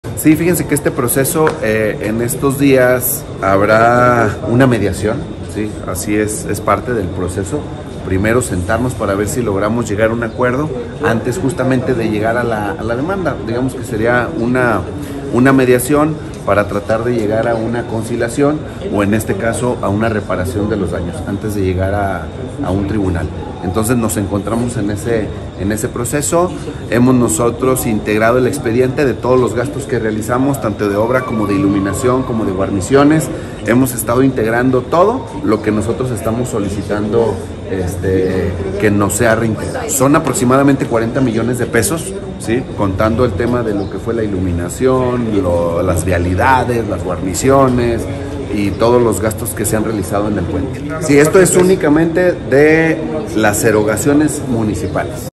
Aseguradora debe pagar a Municipio 40 mdp por daños causados porde pipa a puente de la México
Aseguradora debe pagar a Municipio 40 mdp por daños causados tras incendio de pipa en puente de la México

* Próxima semana se llevará a cabo audiencia de mediación

Aguascalientes, Ags. 27 mayo 2023.- El Municipio de Aguascalientes busca llegar a un acuerdo por 40 millones de pesos, que deberá pagar la aseguradora de la empresa propietaria de la pipa que se incendió bajo el puente de la colonia México, tras ser embestida por un tren carguero, dio a conocer el alcalde de Aguascalientes, Leonardo Montañez Castro.
En entrevista colectiva, explicó que este pago se aplicaría exclusivamente a los gastos realizados por el Ayuntamiento de Aguascalientes, independientemente de aquellos que deban ser cubiertos al Gobierno del Estado, a la empresa Ferromex y a particulares.
El 20 de octubre de 2022 una pipa carga con diesel, cuyo conductor no se detuvo, fue embestida por un tren carguero, bajo el puente vehicular de la colonia México, provocando un fuerte incendio que consumió algunas viviendas y vehículos, además de causar un fuerte daños estructural al paso vehicular que encontraba en reparación y que obligó a la reparación integral.
Montañez Castro aseguró que la audiencia de medición es un primer encuentro encaminado a tratar de llegar a un acuerdo que evite entablar una demanda penal para exigir por esa vía el pago de los daños.
El chofer de la pipa, responsable del hecho, sigue preso.